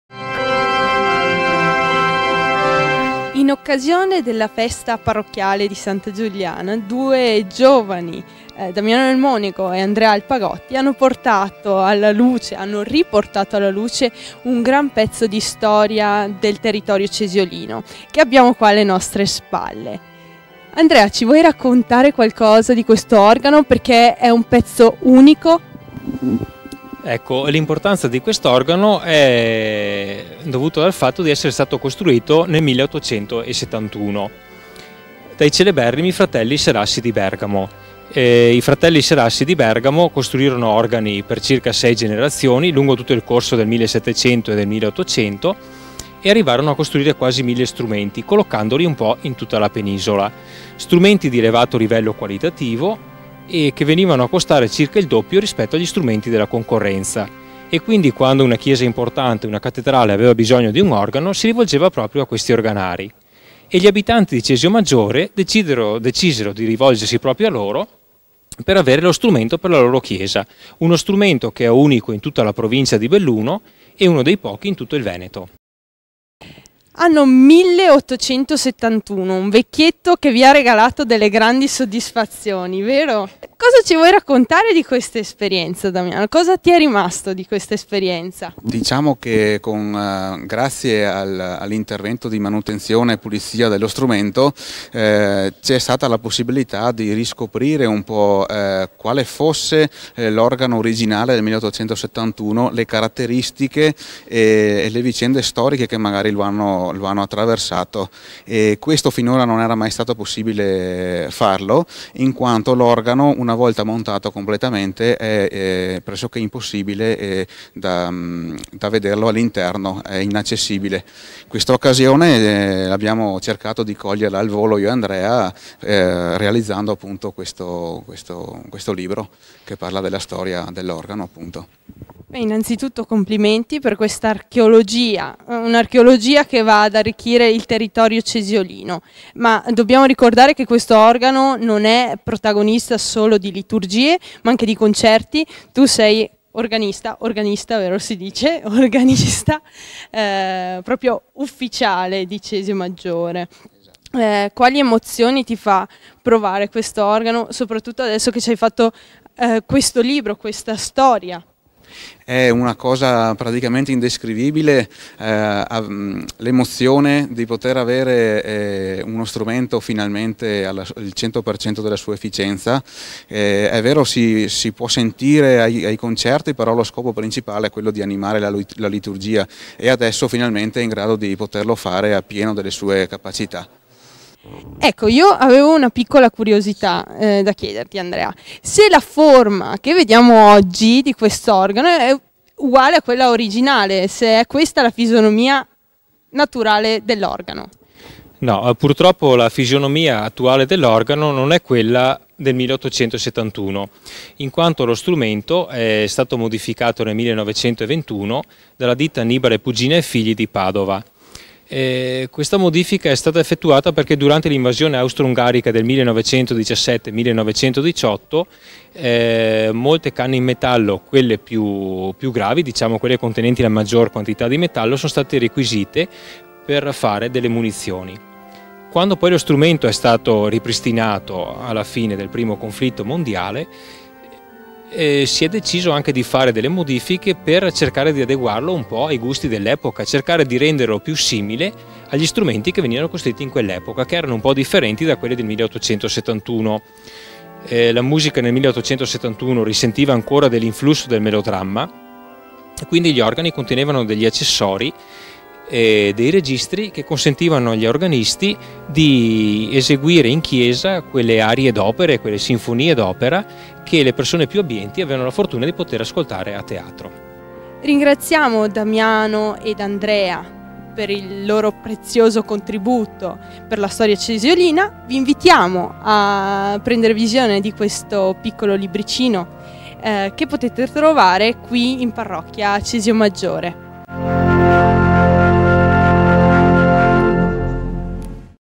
Presentazione libro: "L'organo della chiesa arcipretale di Cesiomaggiore"
Una documentazione storica importante racchiusa nel libro "L'organo della chiesa arcipretale di Cesiomaggiore" con la ricostruzione storica dell'organo della chiesa dalla sua costruzione fino all'ultimo restauro nei giorni nostri.